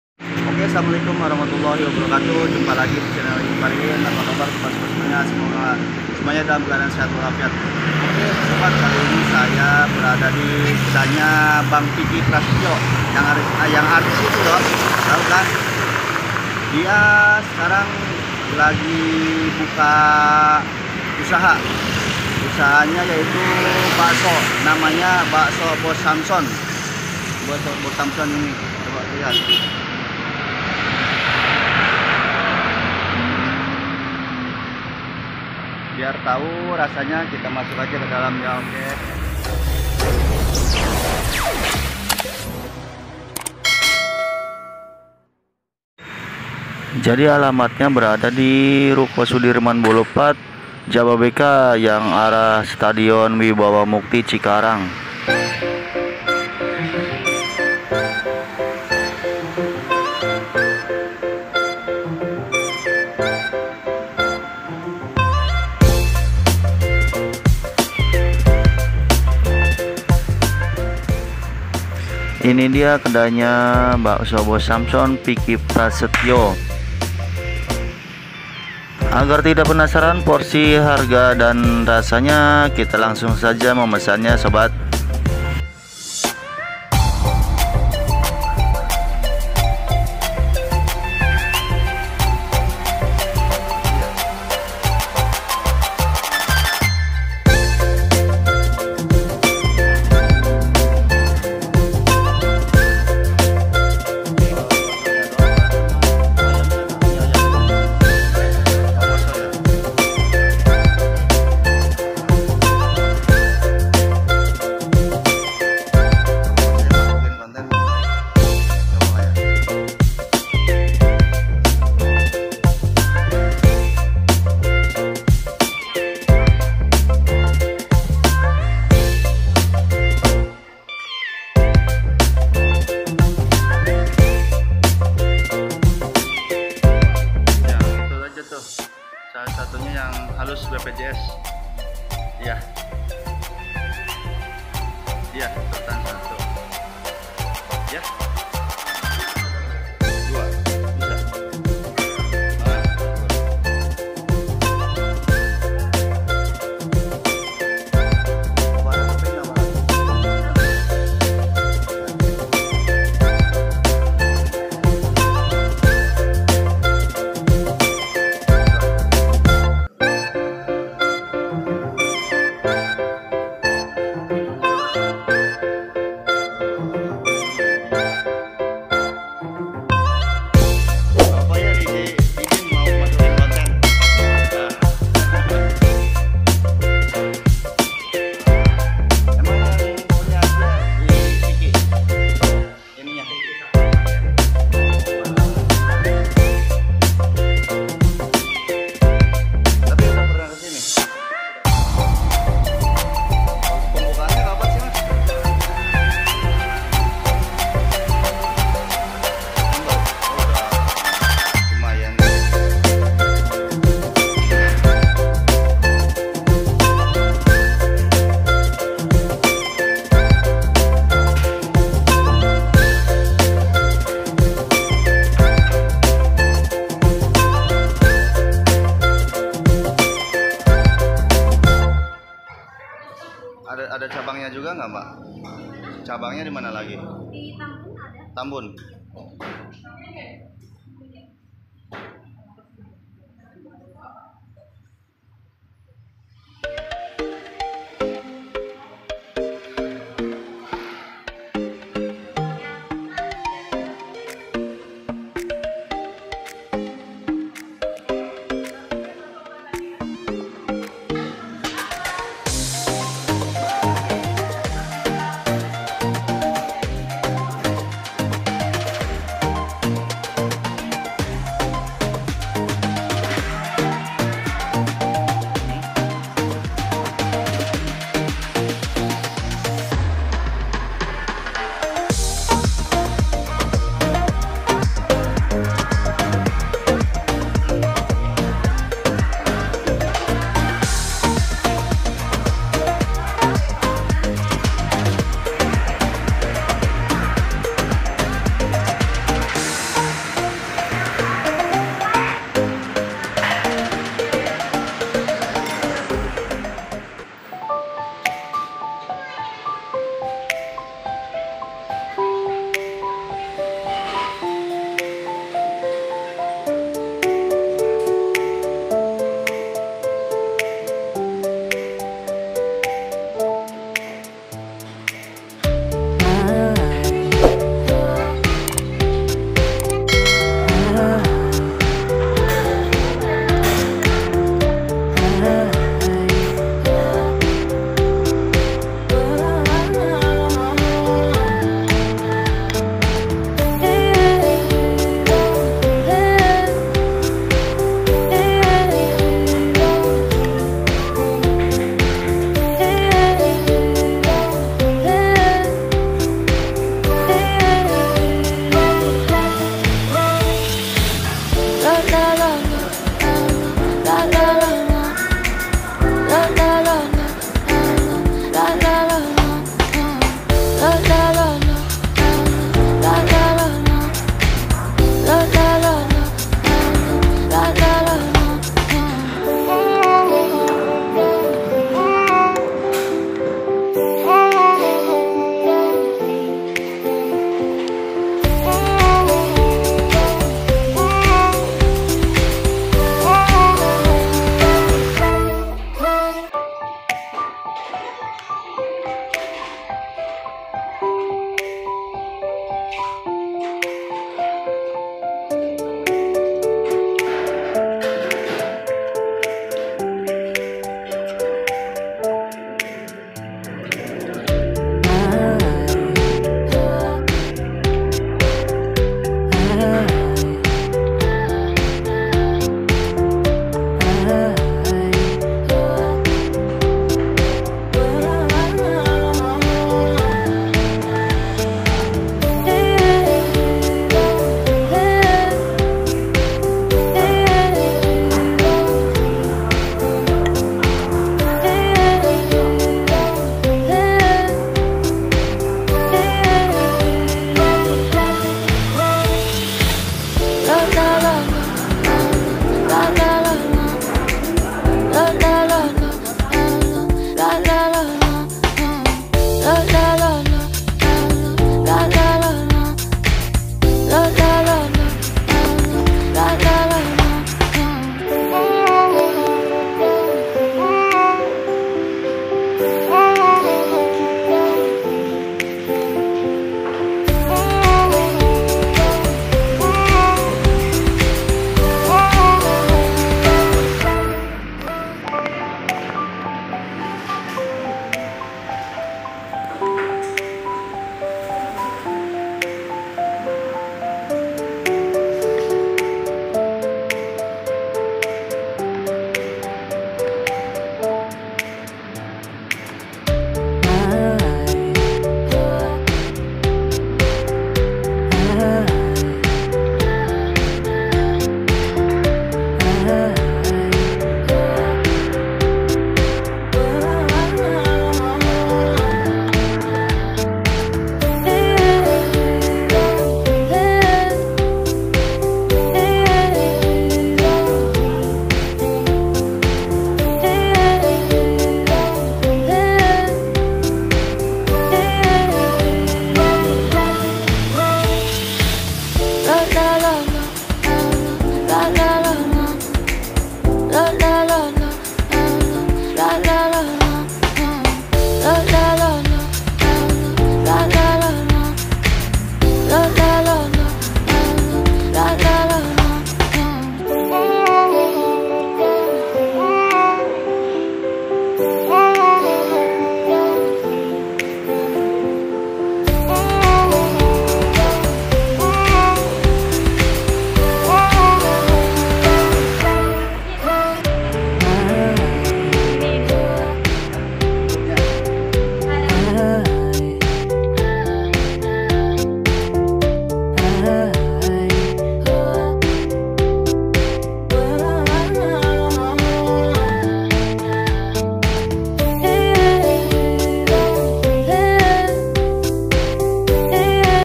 Bakso bos samson Vicki Prasetyo cabang Jababeka Cikarang
sobat @iingparihin535 dicikarang ada kedainya Abang @roni.hermana  nih yang usahanya didunia perkulineran yaitu Bakso Bos Samson yang kedainya di ruko Sudirman boulevard blok B2 no.8 Jababeka cikarang Utara kab.bekasi.